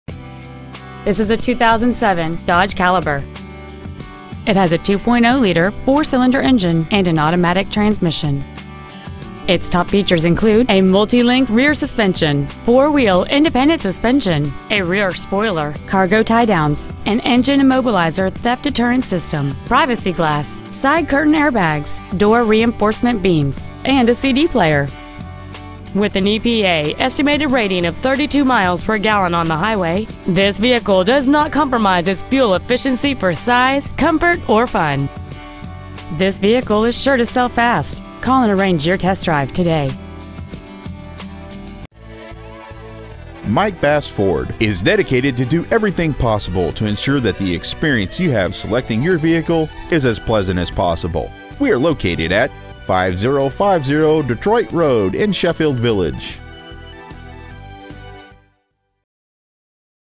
Used 2007 DODGE CALIBER Sheffield Village OH
Year: 2007
 Make: DODGE
 Model: CALIBER
 Engine: 2.0L I4
 Trans.: AUTO
 Exterior: GOLD
 Miles: 94,261
 Interior: TAN

 2007 DODGE CALIBER Sheffield Village  OH